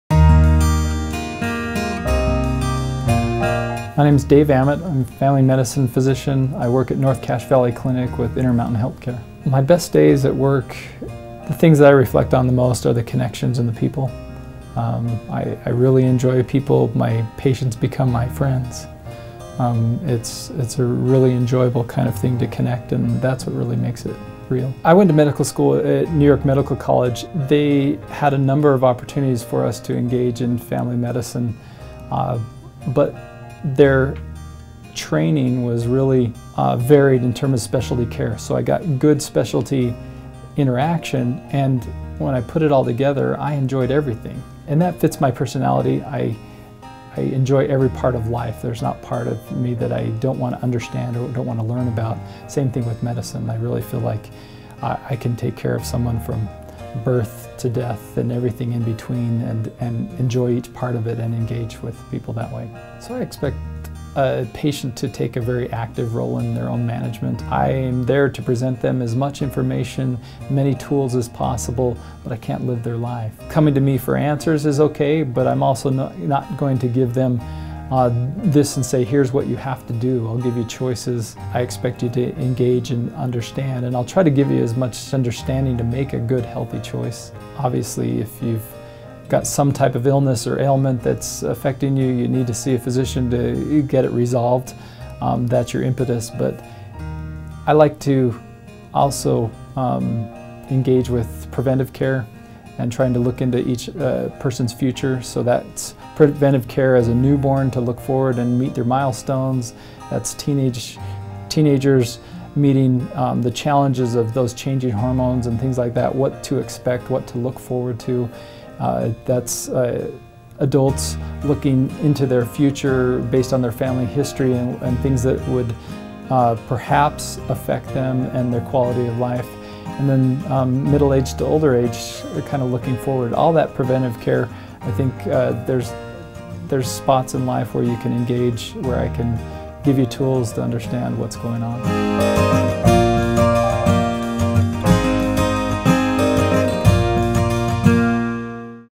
Dr. David S. Aamodt, MD
Dr. David Aamodt, MD, is a primary care physician who practices at the Intermountain Healthcare North Cache Valley Clinic. He is accepting new patients.